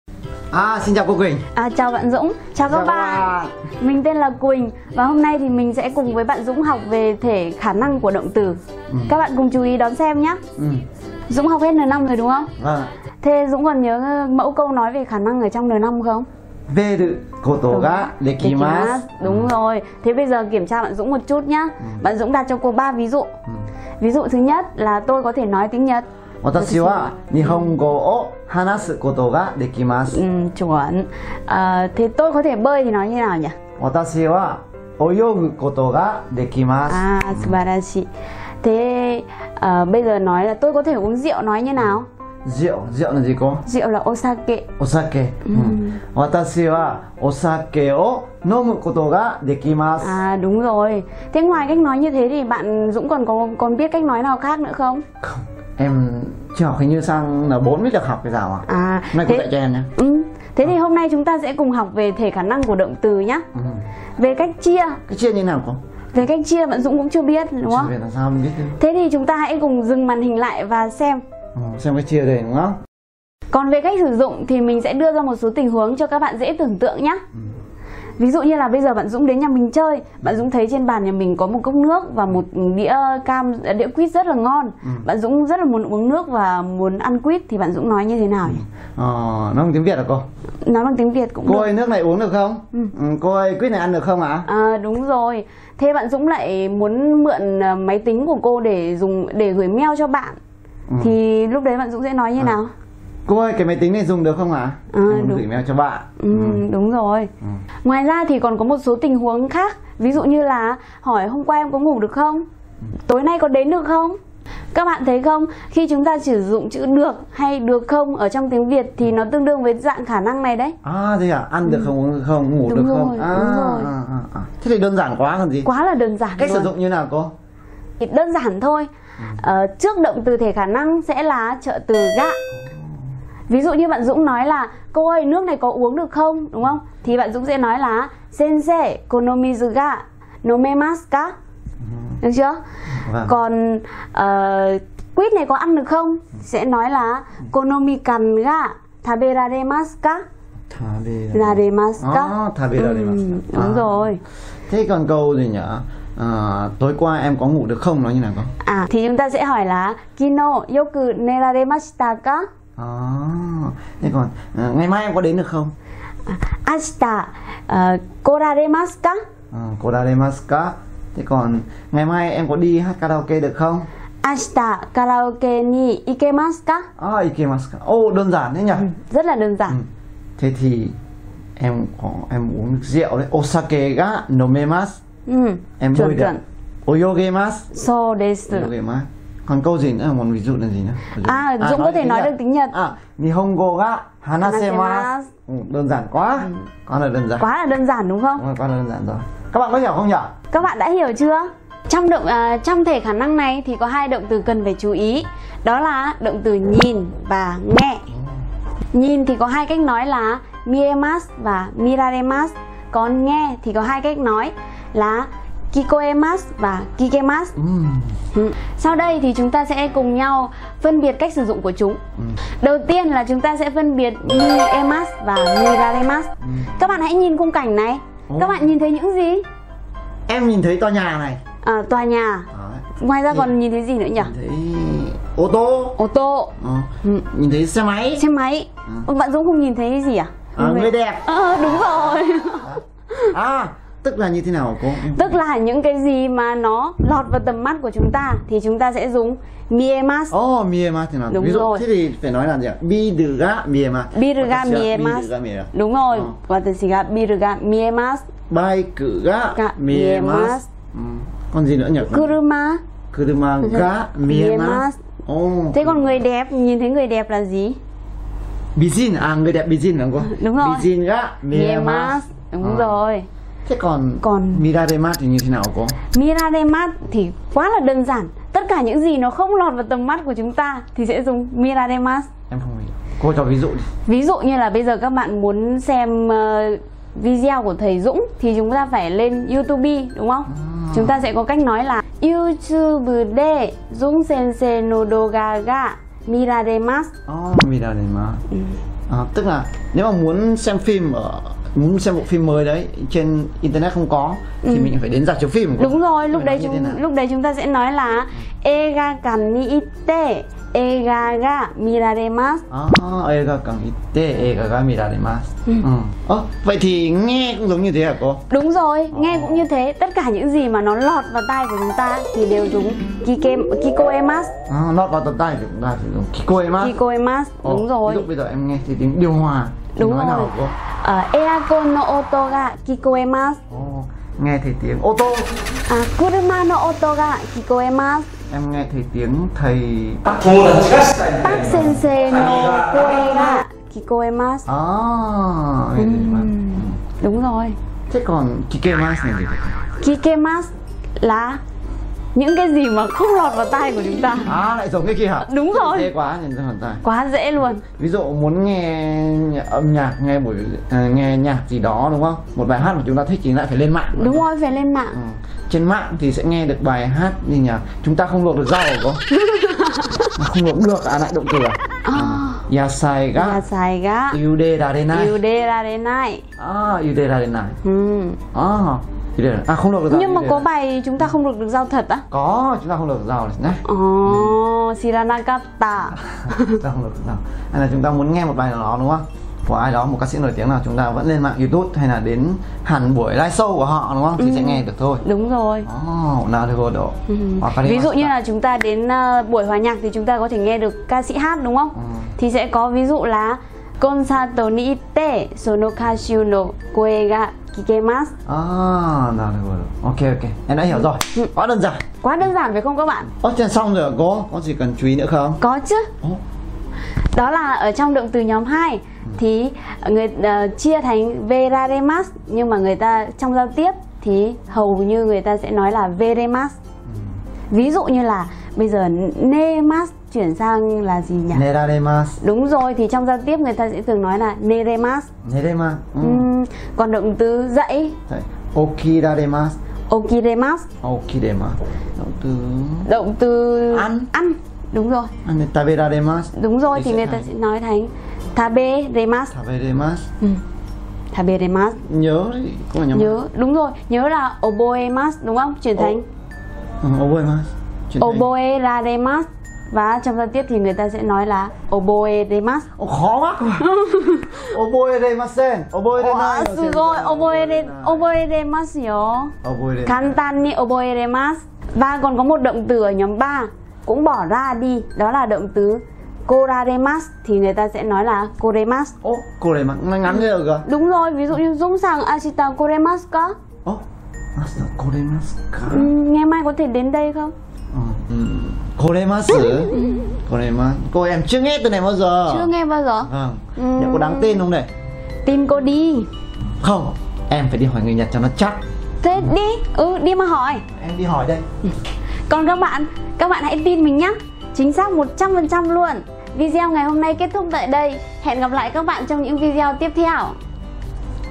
bài 27 mina - Chúng ta không luộc được rau - Xem xong hiểu luôn
Mina thầy Dũng Mori + cô Quỳnh Mori
Ngữ pháp bài 27 mina
1. Thể khả năng:
+ Cách chia thể khả năng
+ Cách sử dụng
2. Cấu trúc  がVthể khả năng
Ý nghĩa: V được/ được không
3. Phân biệt cấc động từ đặt biệt thể khả năng
3.1. Động từ　みます
見えます： chỉ những gì lọt vào tầm mắt
見られます：chỉ những gì không lọt vào tầm mắt
3.2. Nghe 聞きます
聞こえます: những gì lọt vào tai
聞けます: những gì không lọt vào tai
+ Chú ý: Trong văn nói động từ nhóm 2 hầu như nói Vれます
- Đặc biệt trong nhóm 3 từ こられますđược nói thành これます